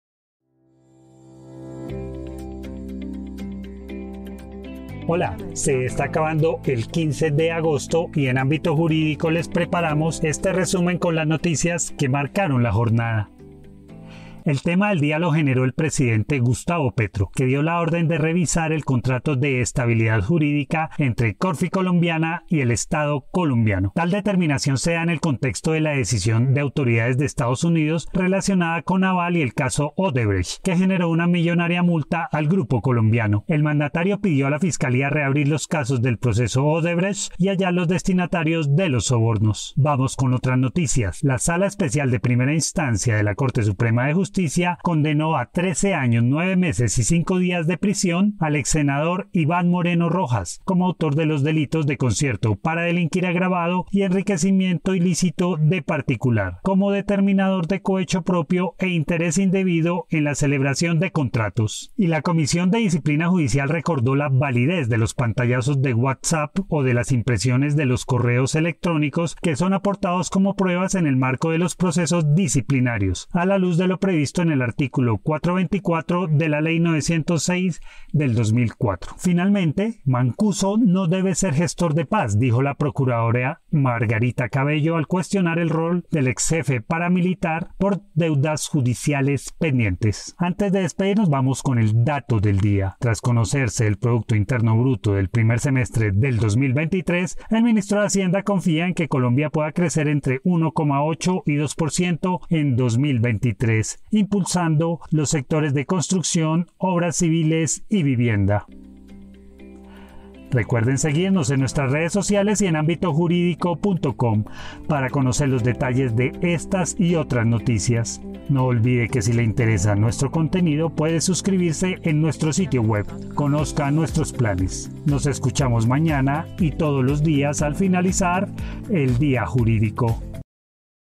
Petro ordena revisar contrato de estabilidad jurídica entre Corficolombiana y el Estado
El mandatario pidió a la Fiscalía reabrir los casos del proceso de Odebrecht; condenan a 13 años al exsenador Iván Moreno Rojas y Procuraduría dice que Mancuso no debe ser gestor de paz, escúchelo todo en El Día Jurídico.